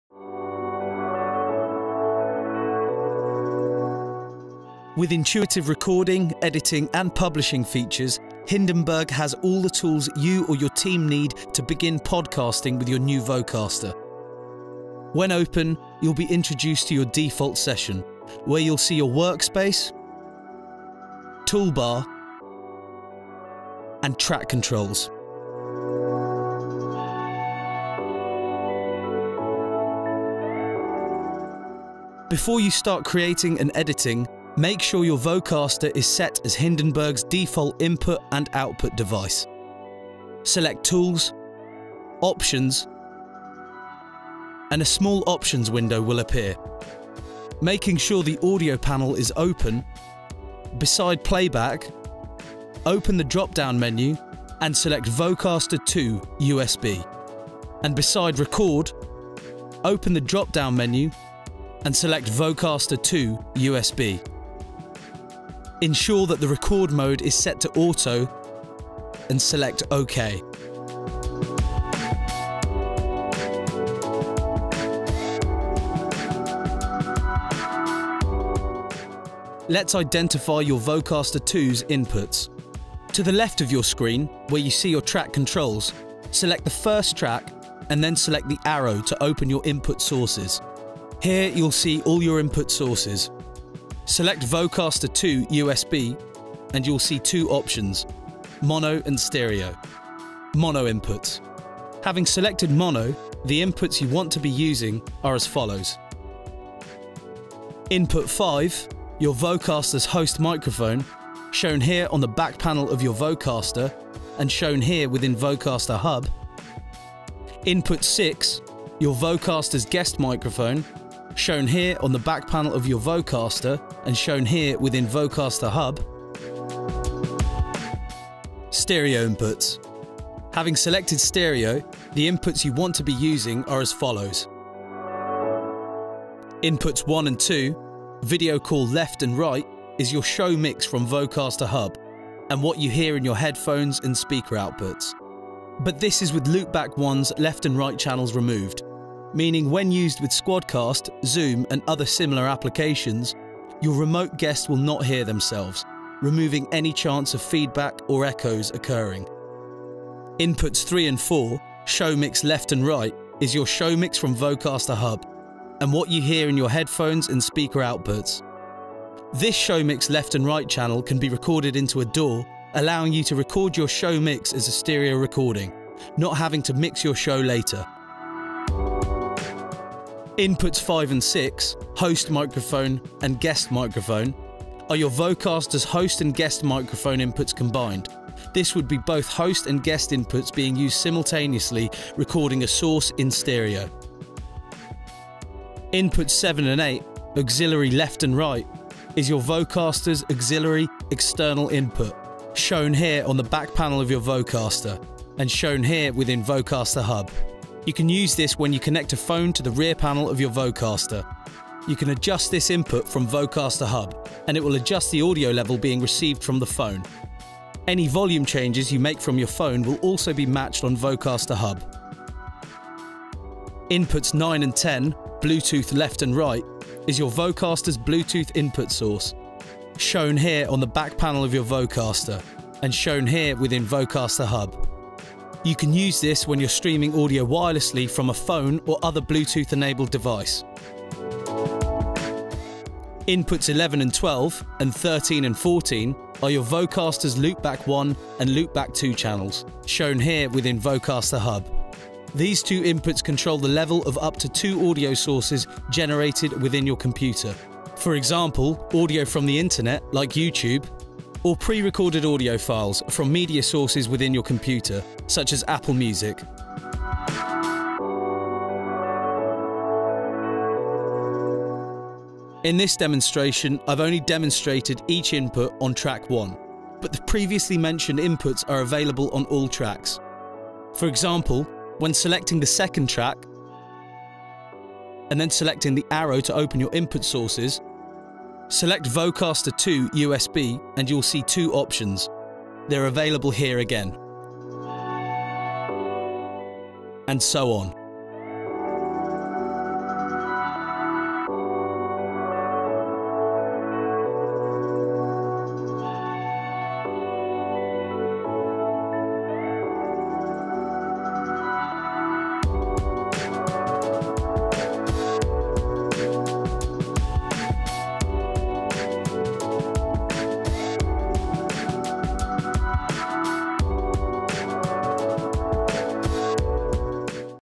Setting up your Vocaster Two in Hindenburg - Windows
In this video, we show you how to set up your Vocaster Two in Hindenburg for Windows, how to find your Vocaster Two's inputs within Hindenburg and where the inputs are on your device and Vocaster Hub.

Chapters:

00:00 - Introduction
00:30 - Audio Settings
01:18 - Finding Your Vocaster Inputs
04:29 - Finding Inputs On Other Tracks